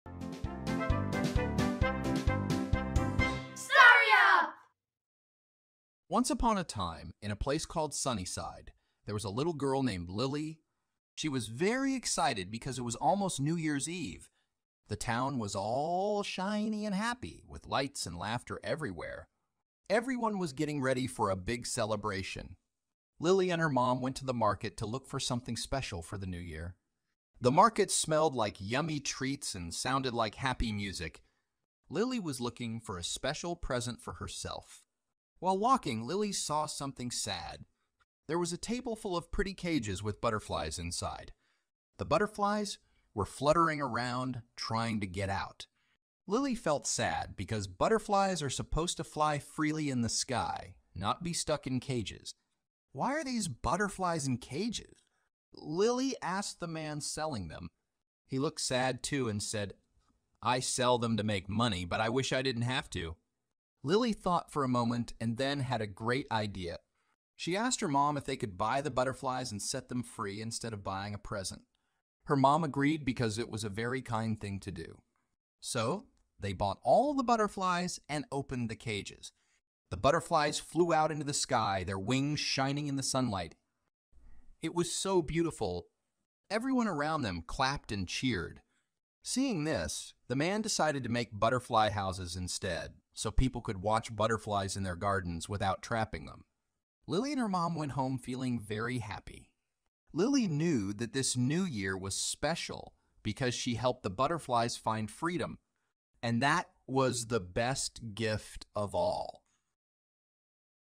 Lily's Butterflies 🦋
✨Join Lily on a heartwarming New Year's adventure in the charming town of Sunnyside! In 'Lily's Wings of Kindness,' witness the magic of a little girl's compassion as she transforms a sad moment into a celebration of freedom.